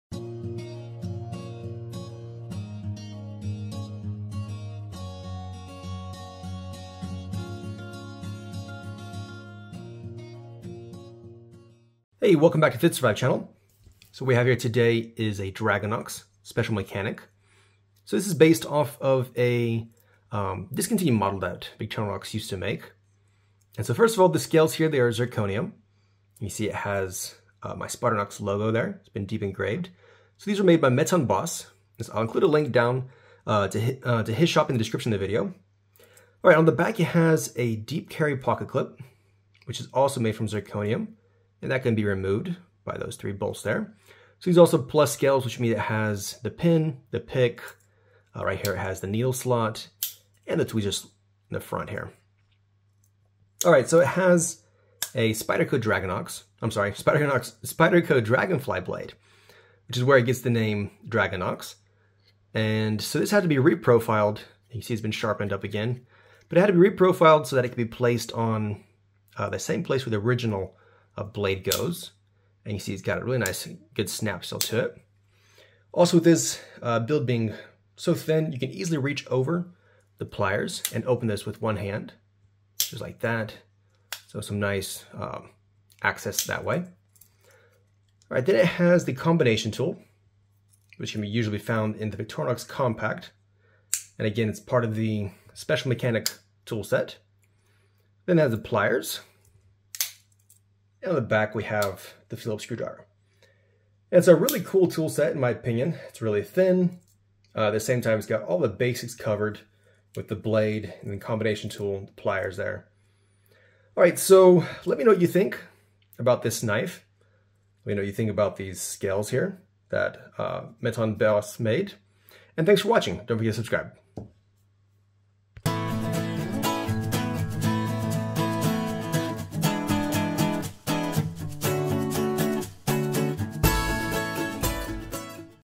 Dragonox Special Mechanic with Zirconium scales!
This toolset seems to be really handy in my opinion.  What do you think about this thin build?  Do you like the look of the zirconium scales with the pocket clip?